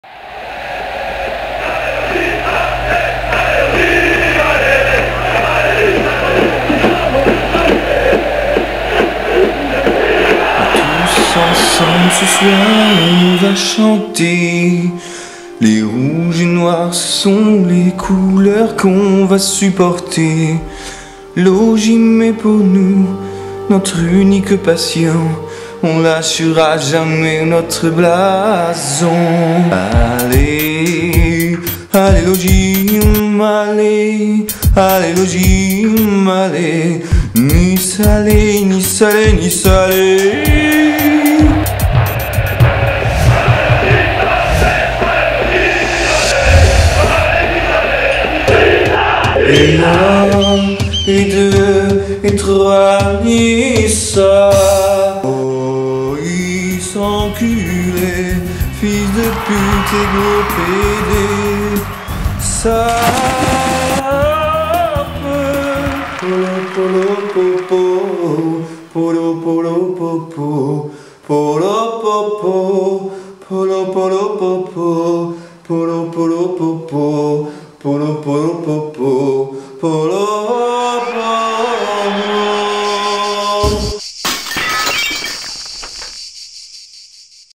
Ti Bolognaise Le Supporter Crooner de Nice
Passionné par l'OGC Nice,Ti Bolognaise se déchaîne dans ce petit clip et met en avant son talent incontestable de crooner... !